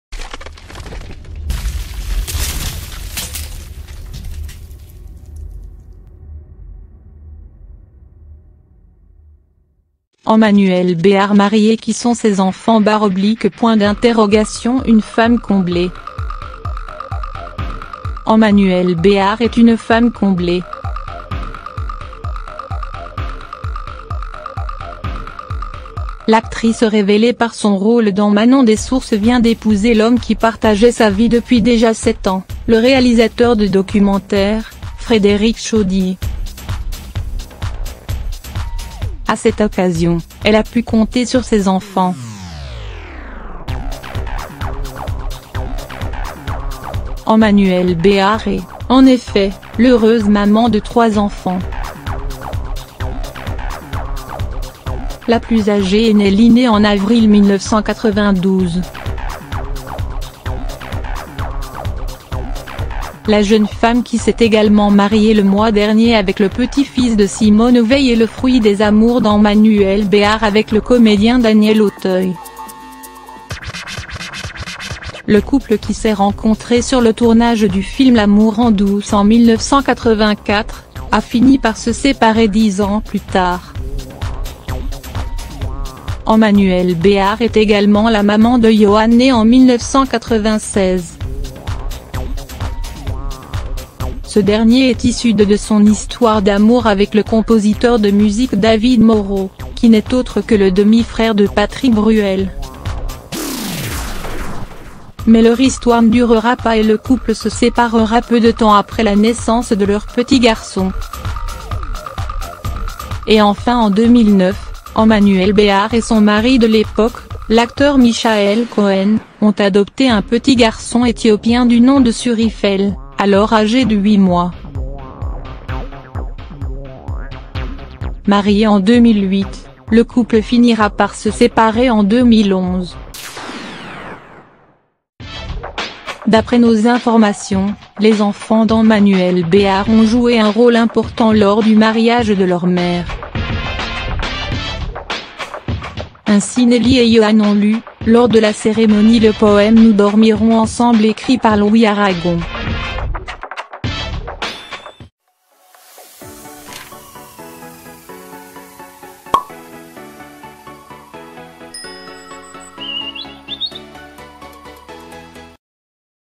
Emmanuelle Béart mariée : qui sont ses enfants ?Une femme comblée
Alors qu'elle s'est mariée ce samedi 22 septembre, Emmanuelle Béart a pu compter sur le soutien de ses trois enfants. Mais qui sont-ils vraiment ?